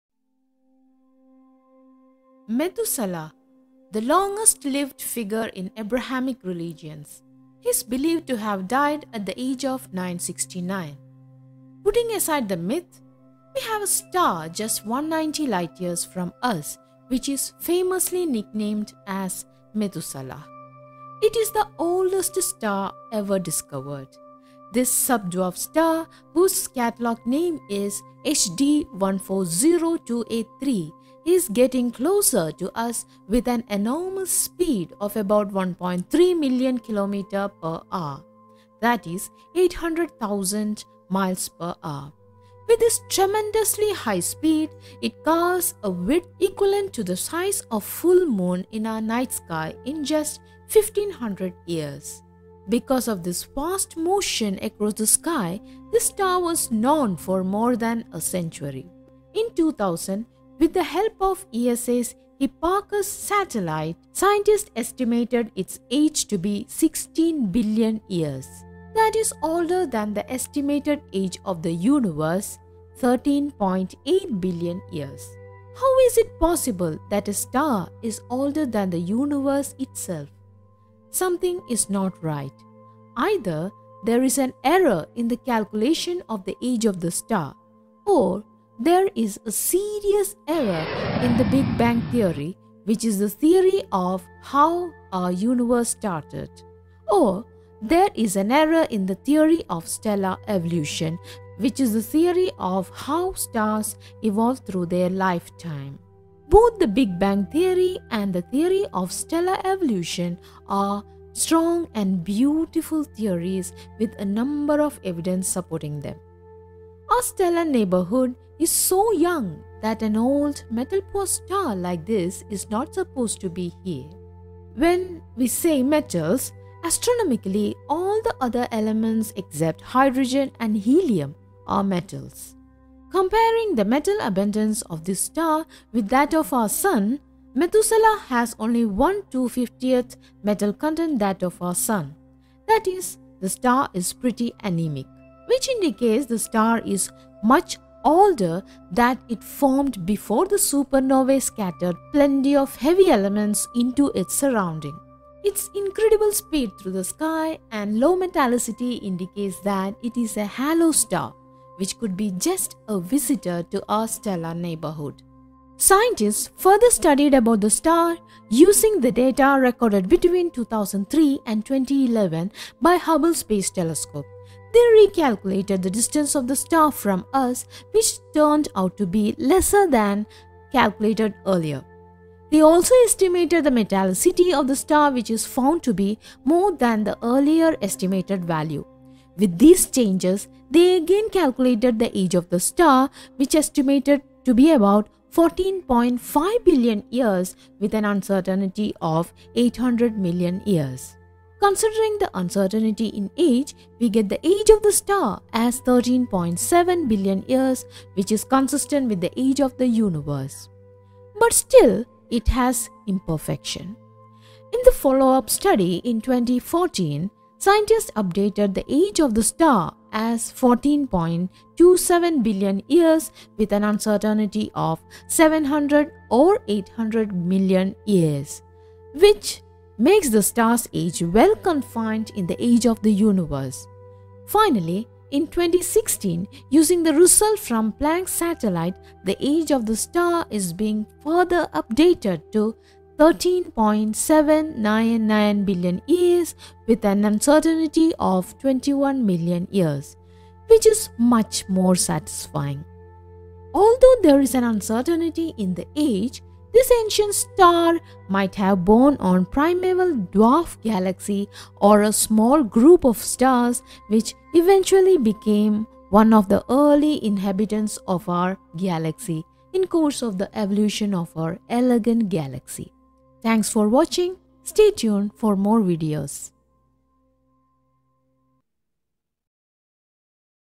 Is the oldest star older than the universe? | Methuselah
Star HD 140283, nick named Methuselah, is the oldest star as we know. In 2000, with the help of ESA’s Hipparcos satellite, scientists found it to be pretty older. Older than the universe itself. Is the star really older than the universe? If it is, then that will challenge the theories that are too beautiful and elegant and all the above which has strong supporting evidences.

Written, Narrated and video editing by:  Nadia A